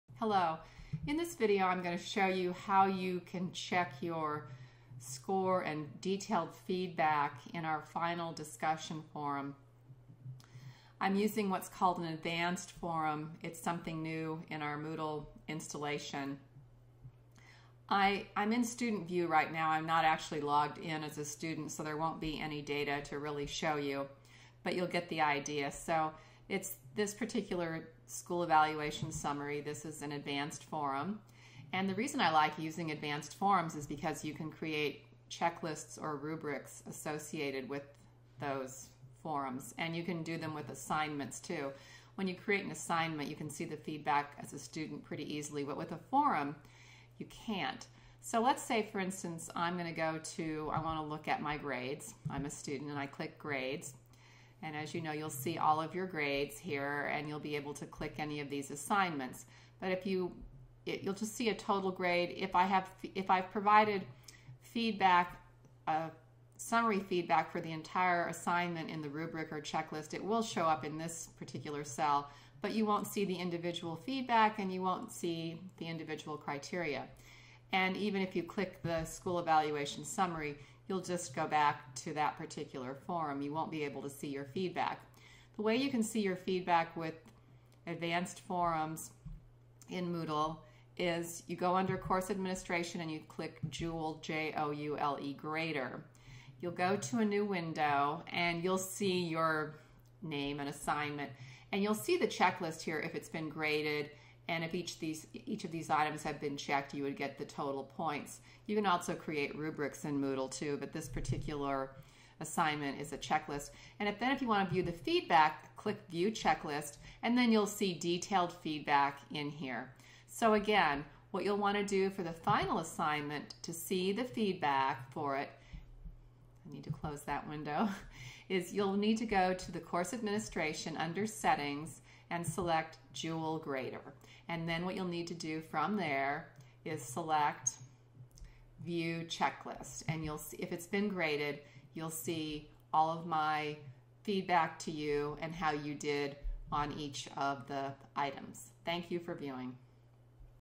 Viewing Feedback Advanced Moodle Forum
See how to view detailed feedback on rubric or checklist in advanced forum in Moodle.